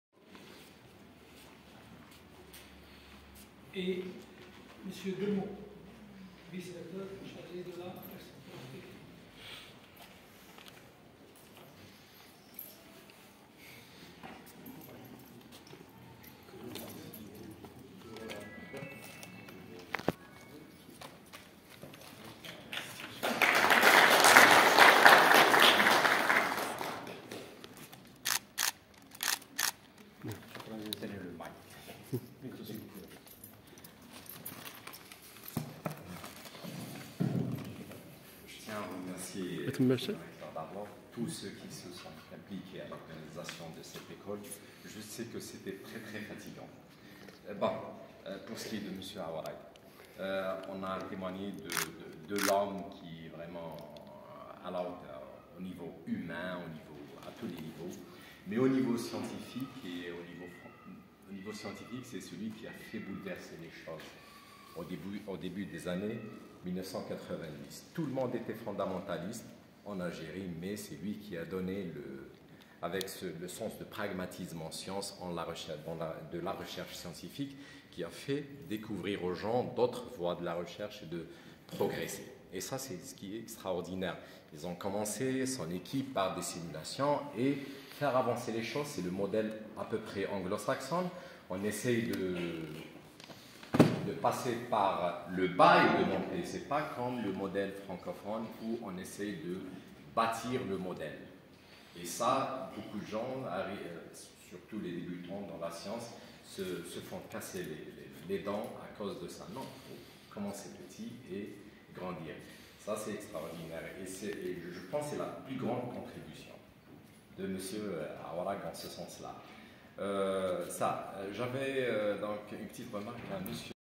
أيام تكوينيةوطنية في دورتها ال 25 في لينيكس البرمجيات الحرة بجامعة بوشعيب بلحاج 18_06_2022
زيارة البروفسور عبد الحفيظ أوراڤ المدير العام السابق للبحث العلمي و التطوير التكنولوجي لجامعة عين تموشنت بلحاج بوشعيب و قدم محاضرة تحت عنوان " الرياضيات التطبيقية كما لم تسمع بها من قبل " يوم الخميس 16 جوان 2022. و قام البروفسور زيادي عبد القادر مدير الجامعة بتكريم كل من البروفسور اوراق عبد الحفيظ، البروفسور مغاشو مراد رئيس جامعة تلمسان و البروفسور رويسات  Ramdane Mezada